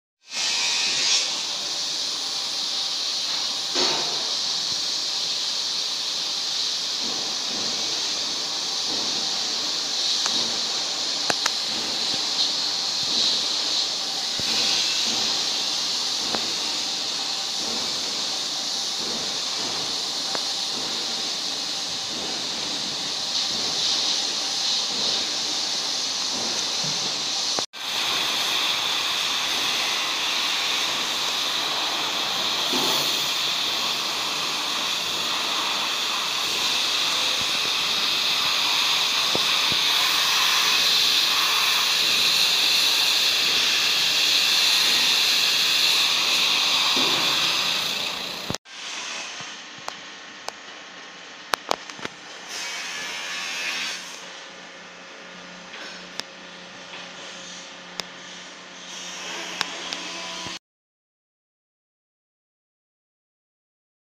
полировка пасти льва
парень полирует лик беттонного льва